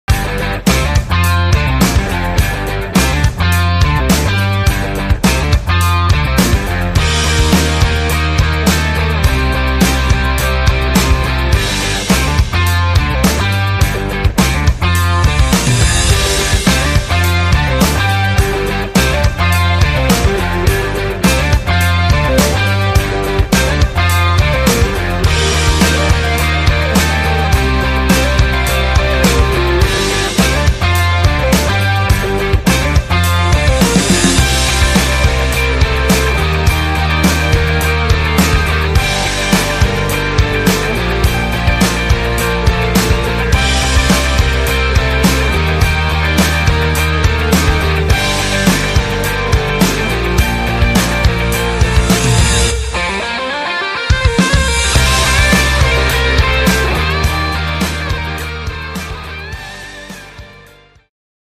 Aménager son atelier facilement : armoire sur pieds STANLEY
Installer simplement en quelques minutes l’armoire sur pieds STANLEY. Les meubles métalliques solides et durables offrent un rangement supplémentaire pour tous les outils et objets d’un garage ou d’un atelier.